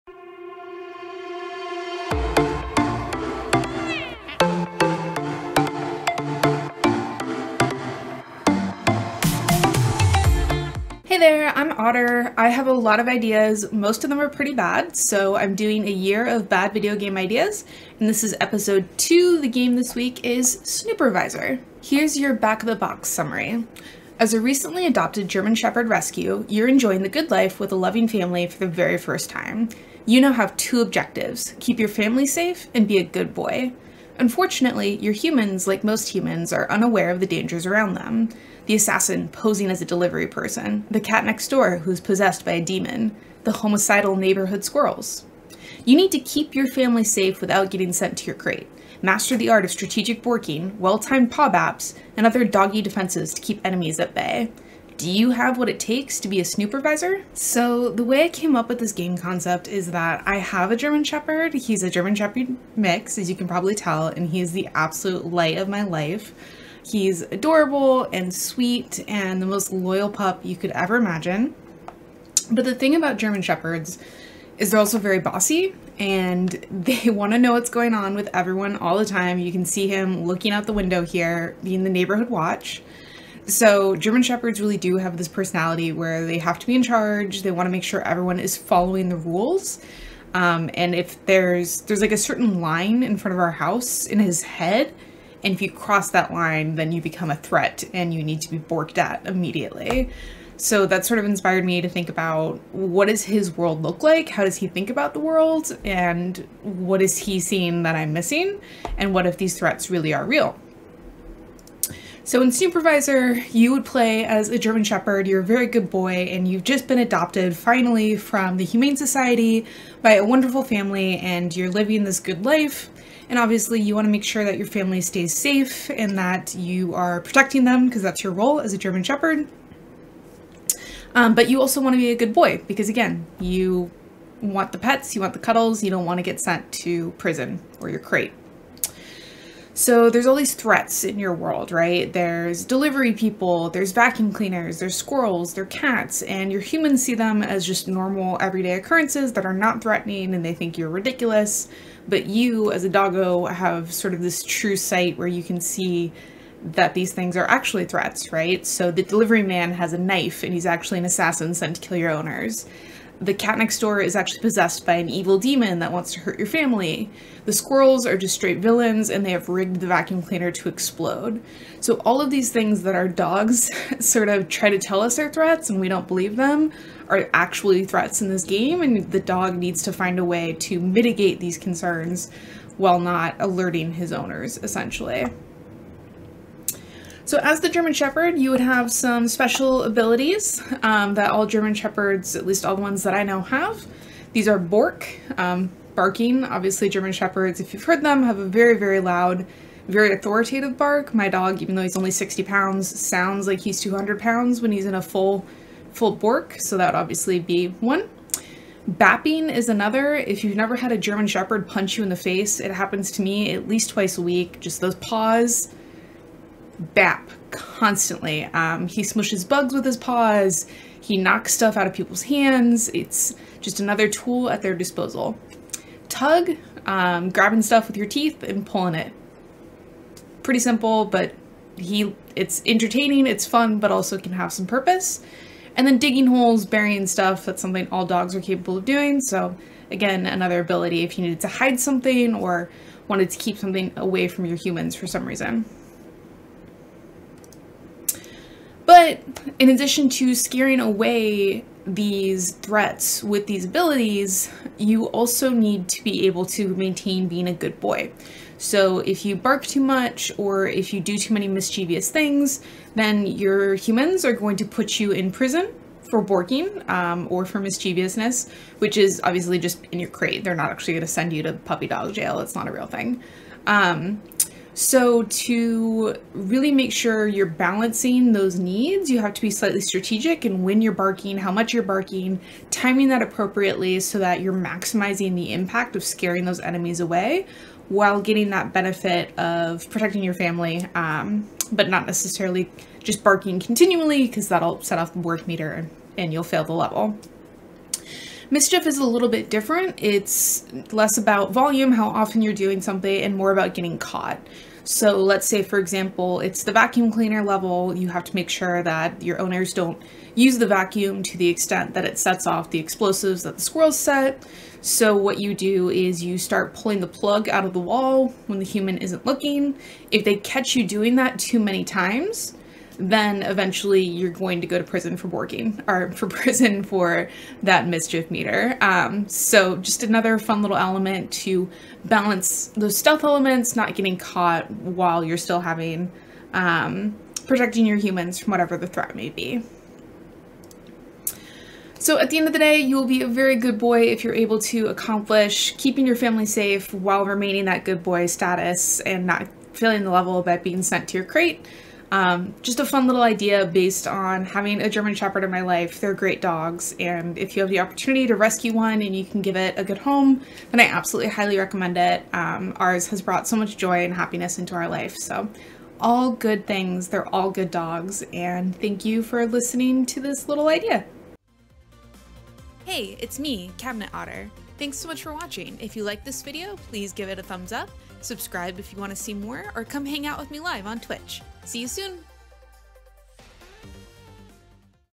Bad Video Game Ideas: Snoopervisor
Episode two in a fifty two episode series of bad video game ideas! I love German Shepherd doggos. I love all doggos, but GSDs have special place in my heart. They're bossy and over protective, but also sweet and good doggos. This game concept stems from my GSD mix trying to protect me from innocuous things like squirrels, cardboard boxes, and junk mail.